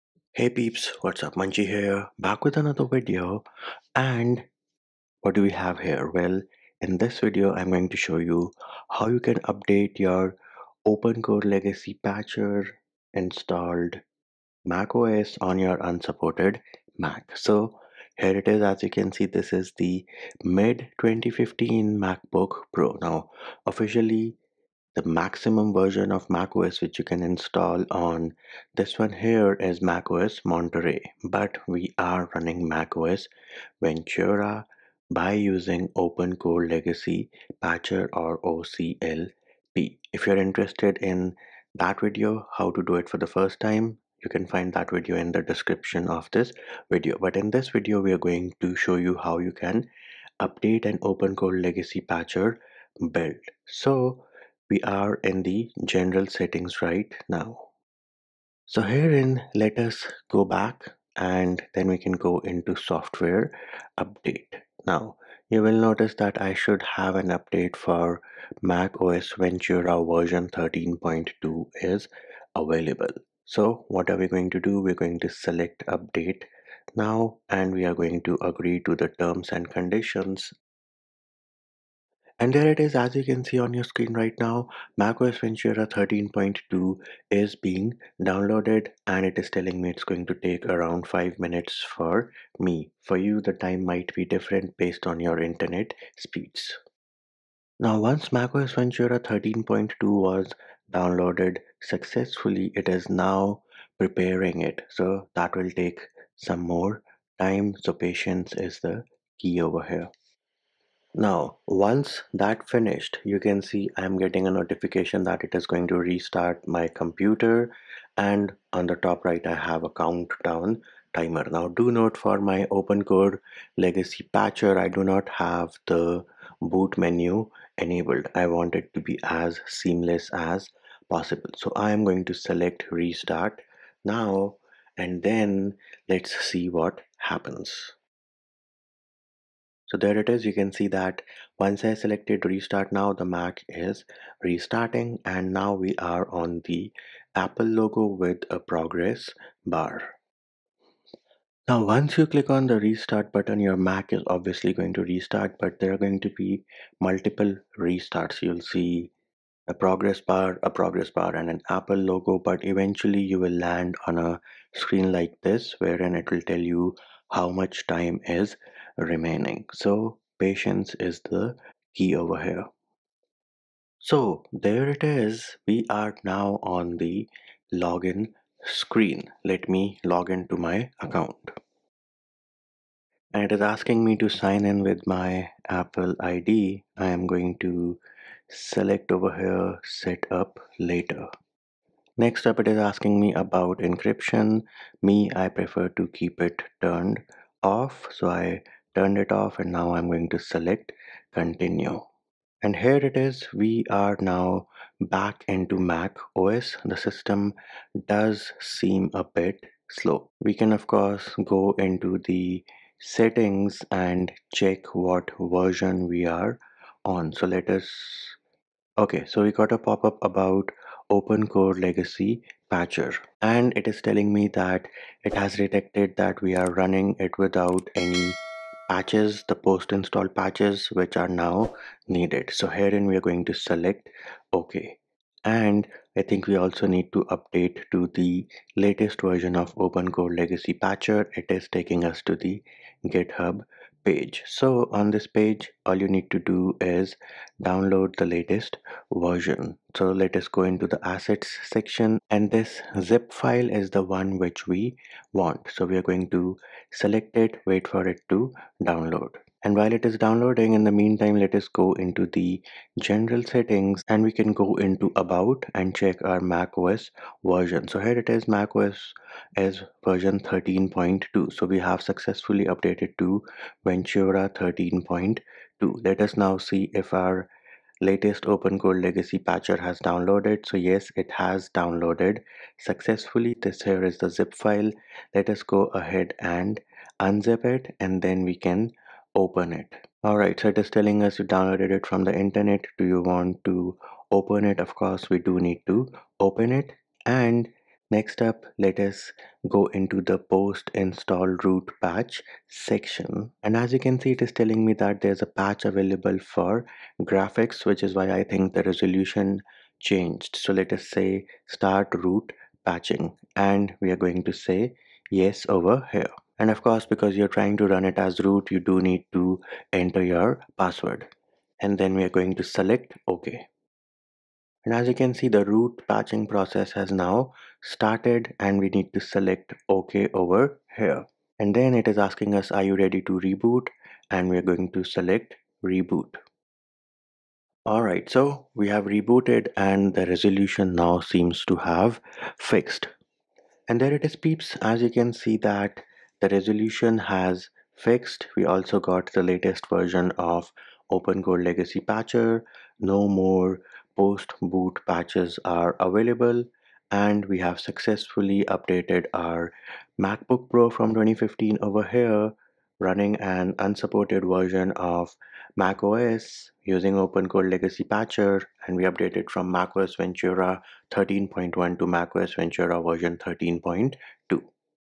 How To UPDATE macOS Ventura on Unsupported Mac With OpenCore Legacy Patcher | DETAILED 2023 Guide
*How To UPDATE macOS Ventura on Unsupported Mac OpenCore Legacy Patcher | DETAILED 2023 Tutorial*

Do you want to learn how to update macOS Ventura running on Unsupported Mac, Macbook, Mac Mini, iMac using OpenCore Legacy Patcher? In this detailed video tutorial we will show you how to update macOS Ventura running on Unsupported Mac, Macbook, Mac Mini, iMac using OpenCore Legacy Patcher. We will be using 15" mid 2015 Macbook Pro for the purpose of this video but this process will work on all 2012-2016 Macs which can run macOS Ventura unofficially via OpenCore Legacy Patcher.